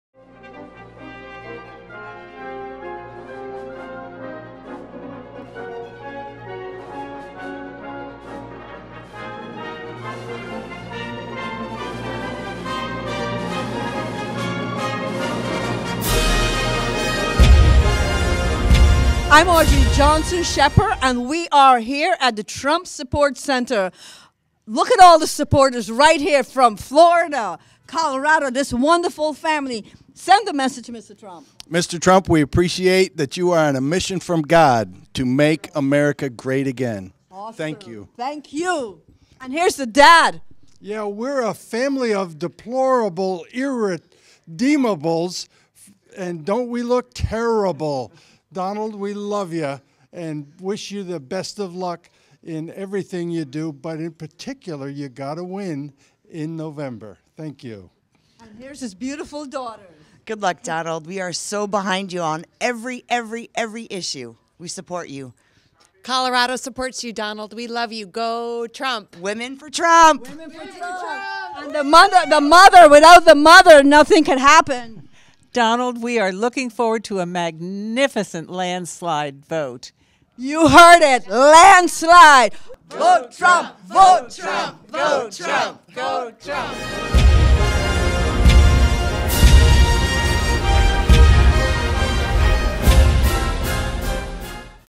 A Landslide win for Donald J Trump
Colorado family supports Donald J Trump for President.